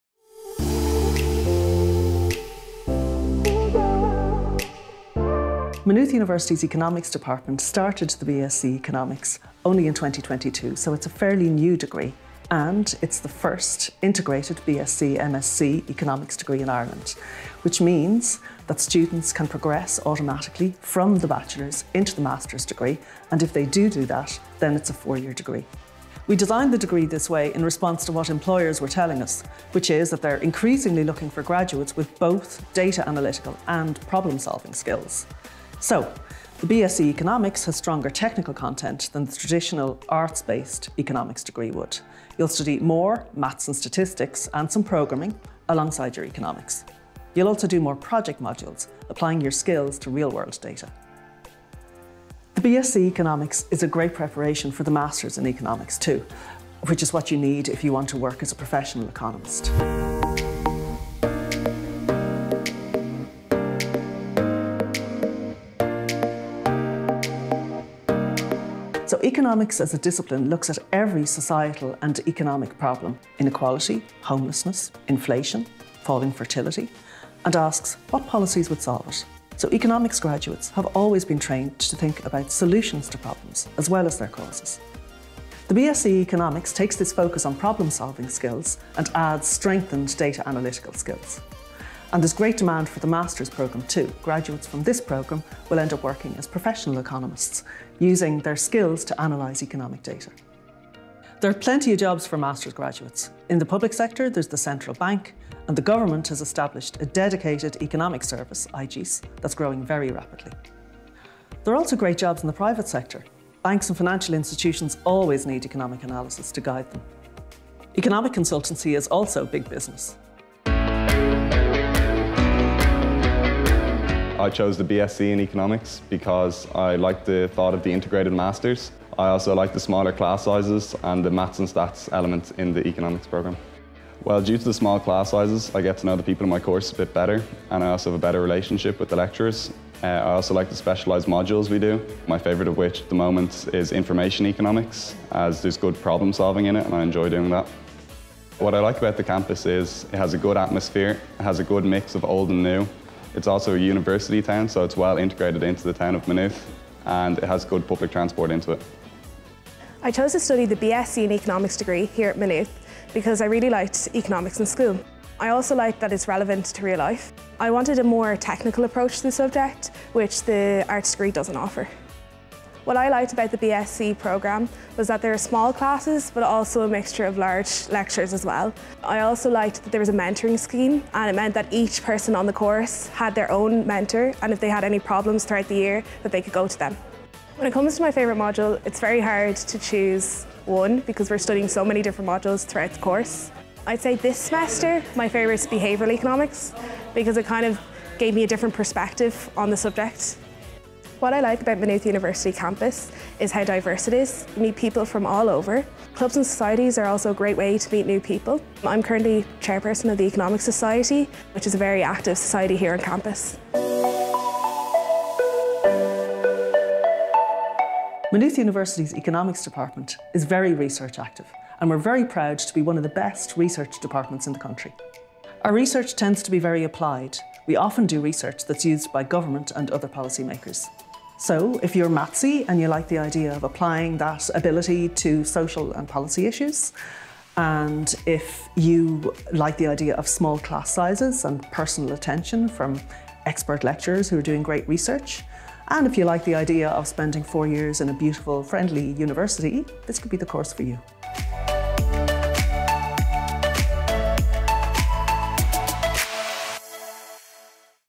Why Maynooth’s BSc Economics could be the right choice for you
If you like maths, enjoy problem-solving, and care about society’s problems, Maynooth’s BSc in Economics could be the right degree for you. Our BSc Economics is integrated with the MSc Economics, providing many rewarding career options, including working as a professional economist. Students have access to top researchers and lecturers in the field, as well as a beautiful campus in a friendly, student-focused university.

Videographer: Daniel Balteanu